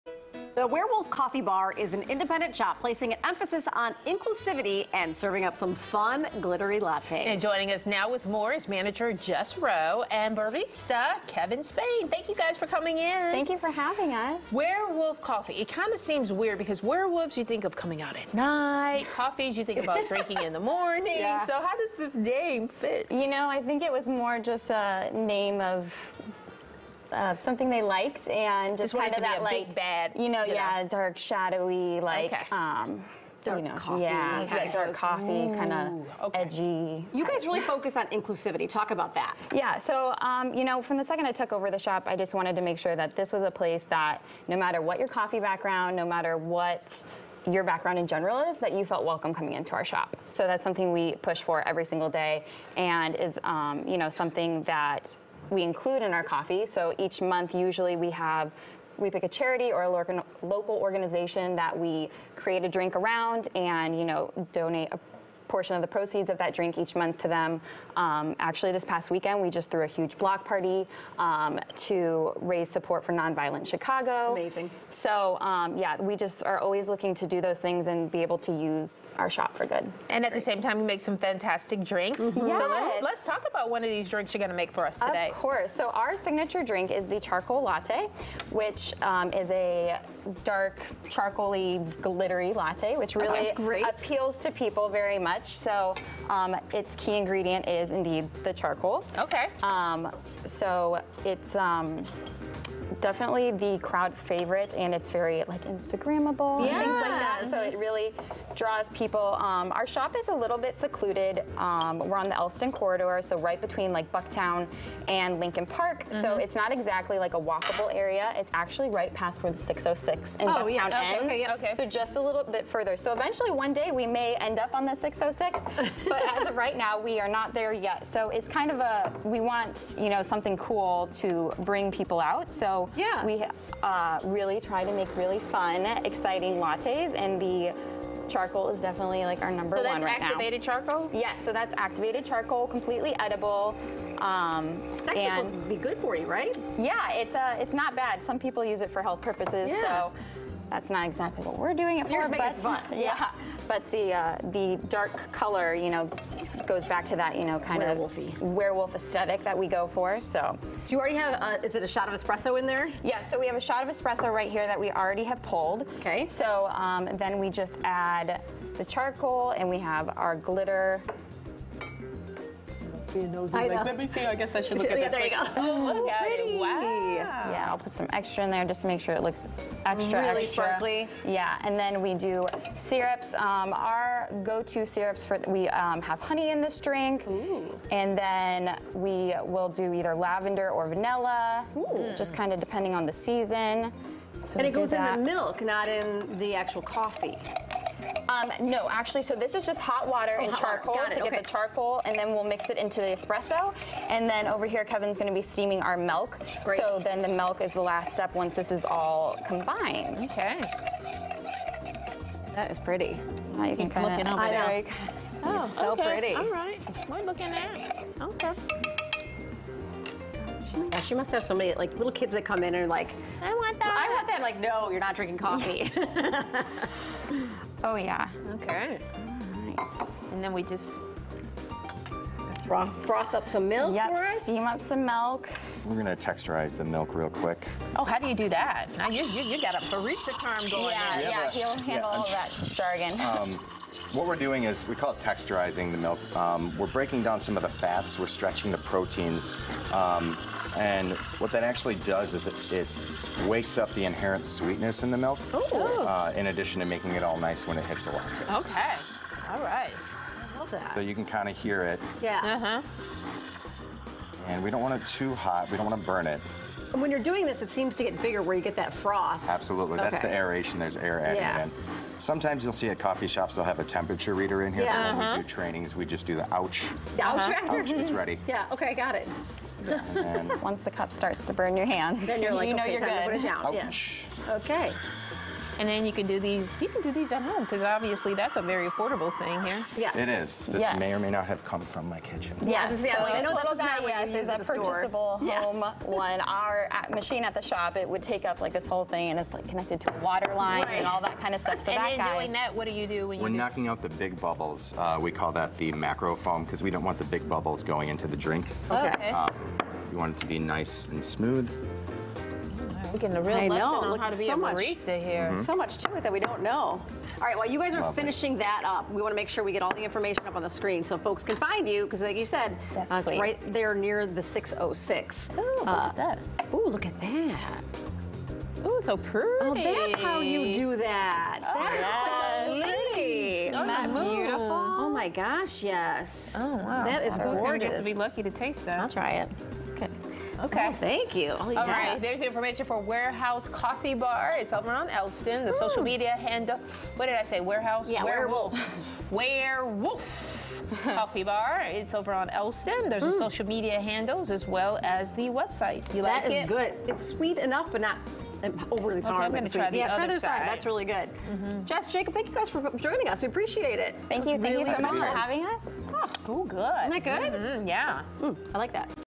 Independent coffee shop placing an emphasis on inclusivity
The Werewolf Coffee Bar is an independent shop placing an emphasis on inclusivity and serving up some fun, glittery lattes. Joining us now with more is manager Jess Roe and barista Kevin Spain.